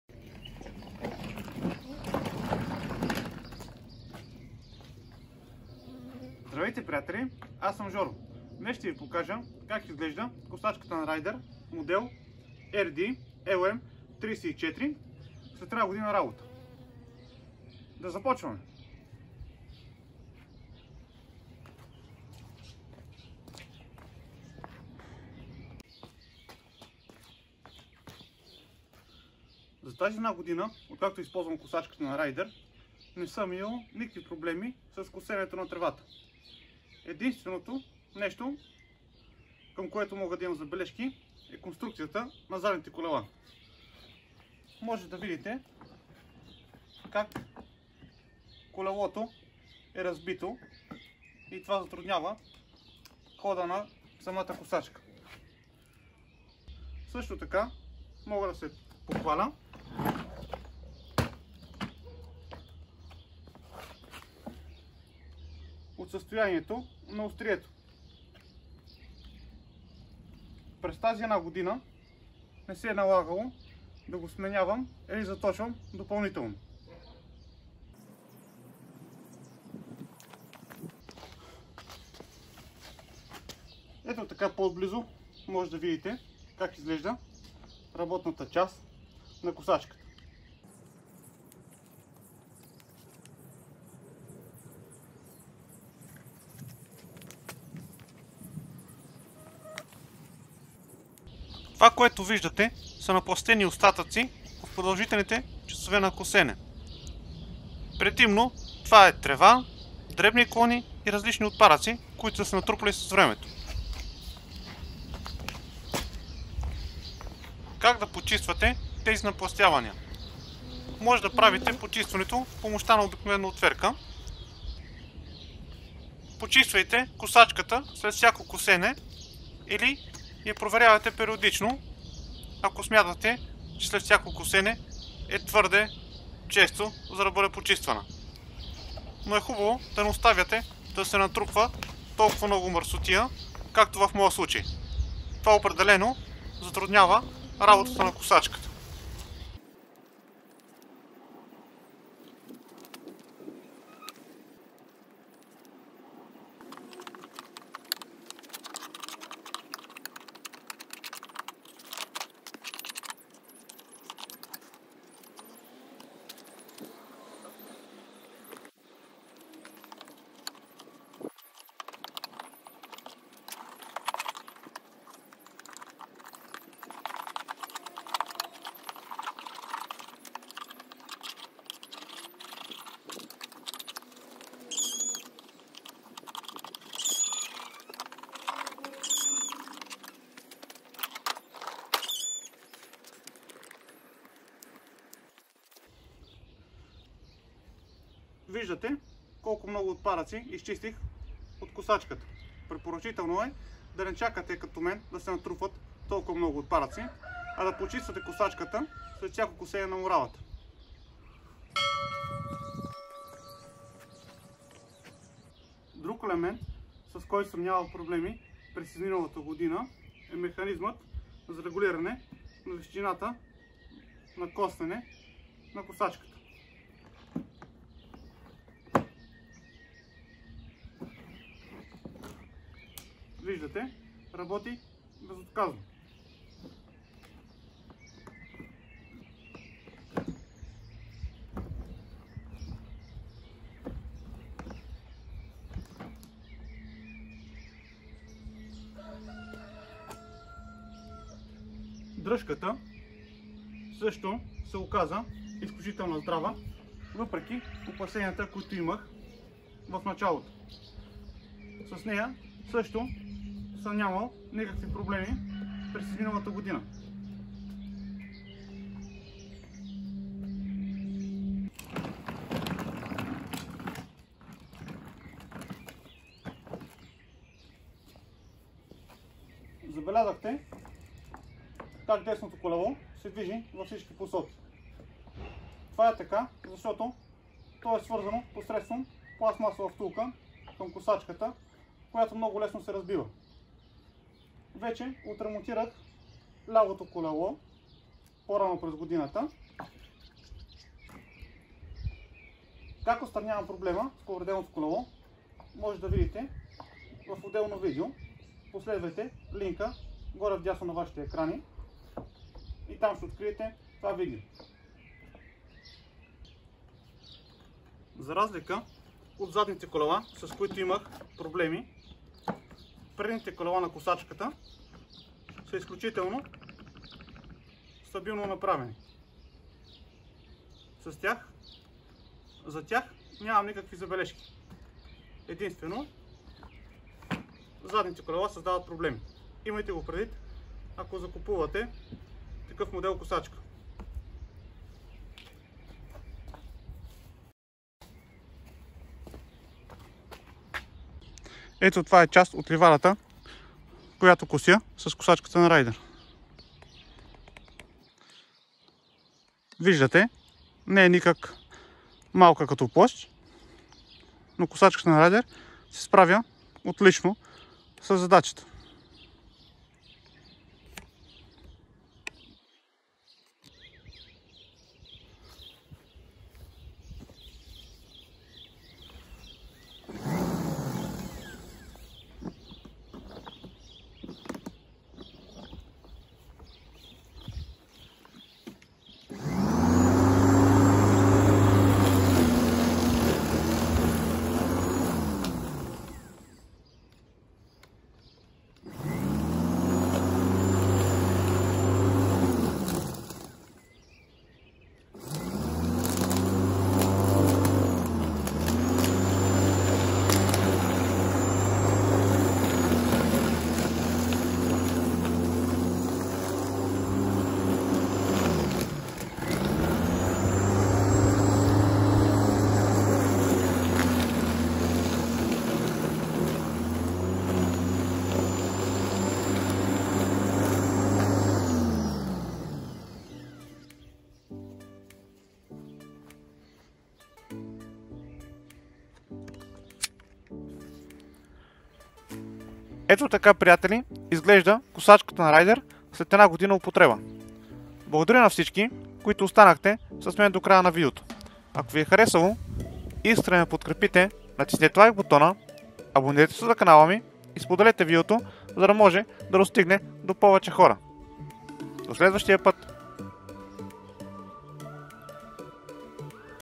RAIDER RD-LM34 - Една година по късно / RAIDER RD-LM34 Electric Mower - One Year Later.
Във видеото може да видите как изглежда електрическа косачка RAIDER RD-LM34 след една година употреба. Разказвам за положителните и отрицателните неща на косачката от опита който имам с нея.

Електрическа косачка RAIDER RD-LM34 :
Номинално захранващо напрежение - 240V
Честота на променливия ток - 50Hz
Мощност, W: 2000
Максимални обороти, min-1: 3300
Работна ширина, mm: 430
Работна височина, mm: 20 - 70
Вместимост на коша, L: 50
Степен на защита: IP24
Тегло, кг.: 14.400
Тегло Нето, кг.: 12.800
Размери: 740 x 480 x 370 mm
Electric lawn mower RAIDER RD-LM34:
Rated supply voltage 240V
Frequency of alternating current 50Hz
Power, W: 2000
Maximum speed, min-1: 3300
Working width, mm: 430
Working height, mm: 20 - 70
Basket capacity, L: 50
Degree of protection: IP24
Weight, kg: 14,400
Net weight, kg: 12,800
Dimensions: 740 x 480 x 370 mm